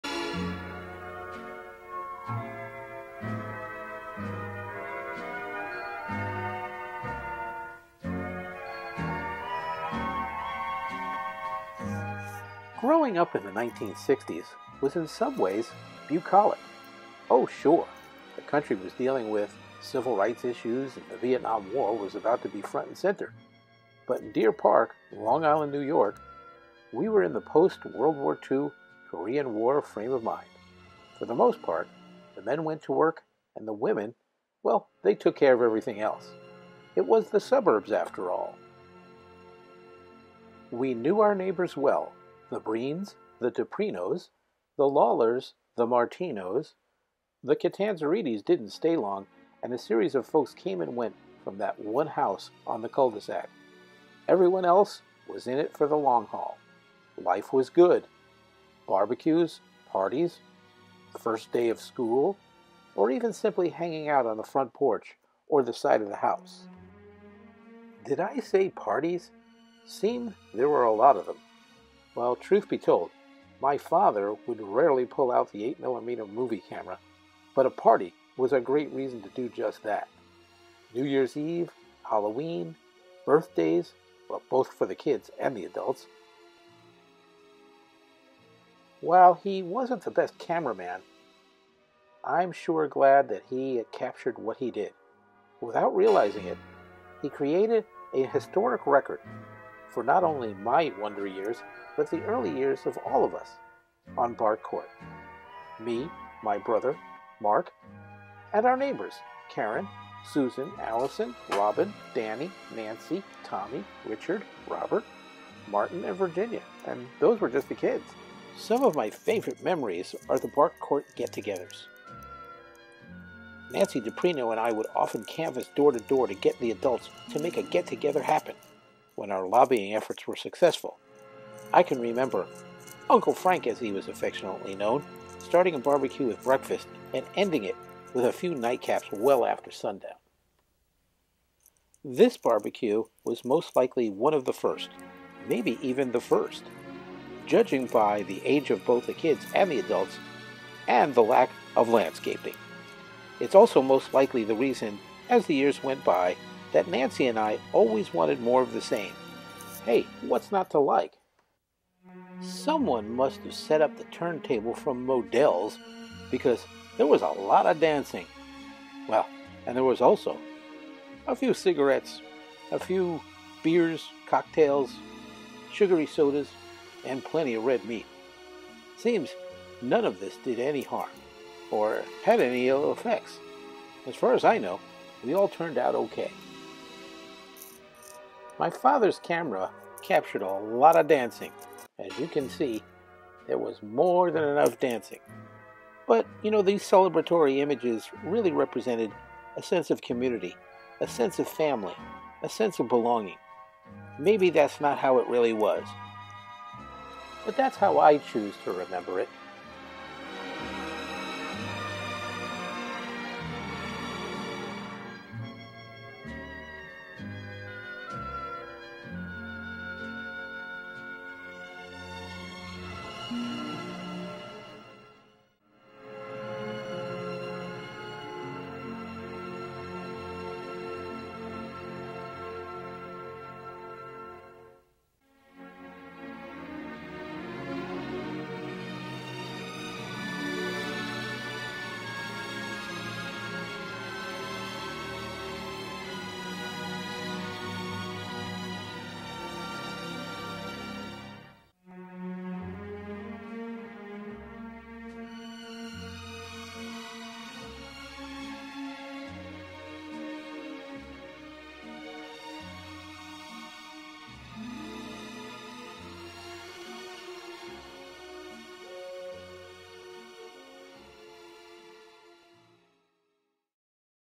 Memories of Bark Court
A look back through home movies at place I called home until I was a teen.